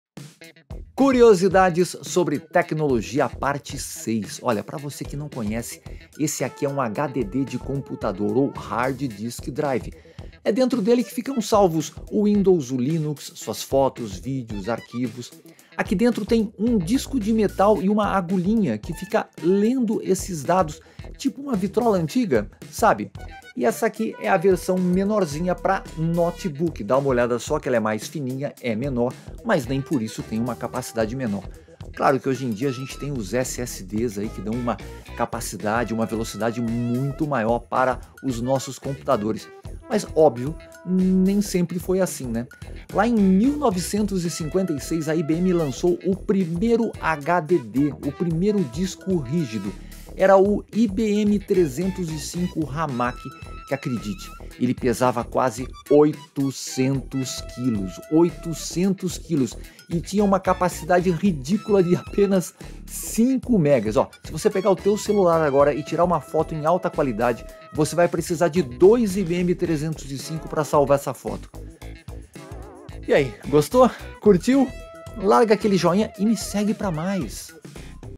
O primeiro HDD para computador
A curiosidade sobre tecnologia de hoje é sobre o primeiro HDD para computador criado pela IBM em 1956, o IBM 305 Ramac, que pesava quase 800 kg e tinha capacidade de armazenamento de apenas 5 MB.

Gostou? Então deixa um Joinha e me segue para mais!